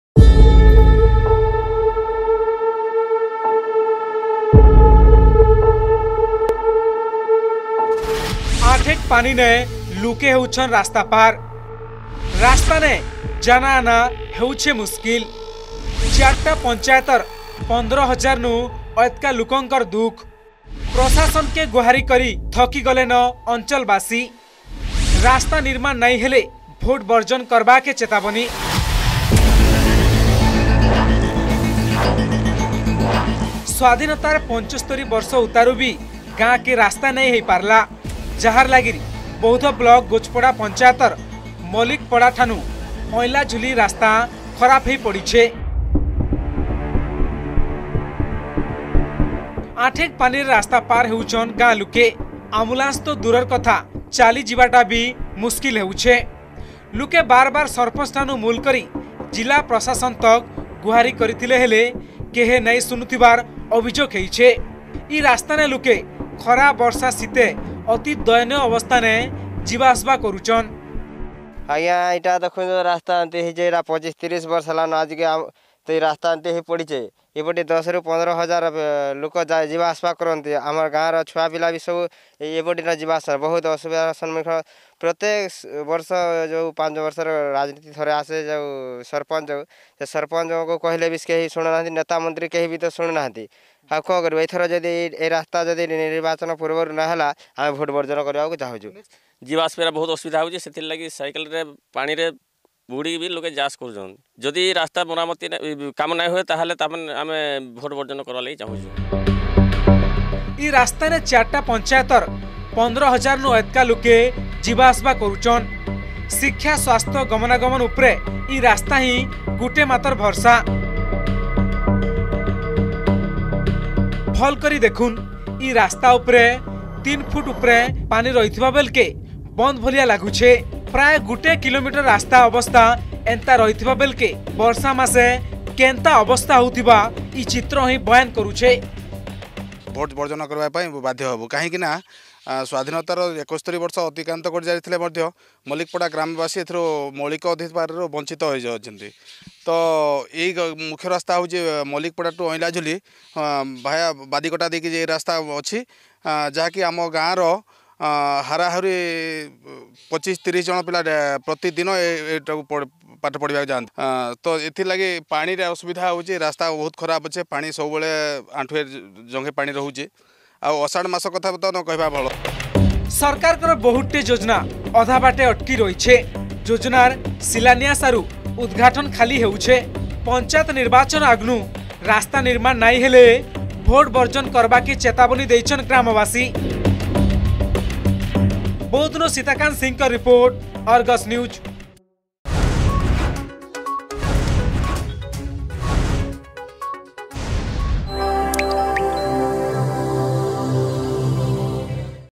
ଆଁଠେକ୍ ପାନିନେ ଲୁକେ ହେଉଛନ୍ ରାସ୍ତା ପାର୍
ଆମ୍ଭୁଲାନ୍ସ ତ ଦୂରର କଥା ଚାଲି ଚାଲି ଯିବା ବି ହେଇଛେ ମୁସ୍କିଲ୍ । ଆଁଠେକ୍ ପାନିନେ ଲୁକେ ହେଉଛନ୍ ରାସ୍ତା ପାର୍ ।  ୪ ଟା ପଂଚାୟତର୍ ୧୫ ହଜାରନୁ ଅଏଧକା ଲୁକ୍ ଇ ରାସ୍ତା ଦେଇକରି ଯିବା-ଆସଵା କରସନ୍। ପ୍ରଶାସନକେ ଗୁହାରୀ କରି ଥକି ଗଲେନ୍ ଅଞ୍ଚଲବାସୀ । ଏବେ ରାସ୍ତା ନିର୍ମାନ ନାଇହେଲେ ଭୋଟ୍ ବର୍ଜନ କରବାକେ ଚେତାବନୀ ଦେଇଛନ୍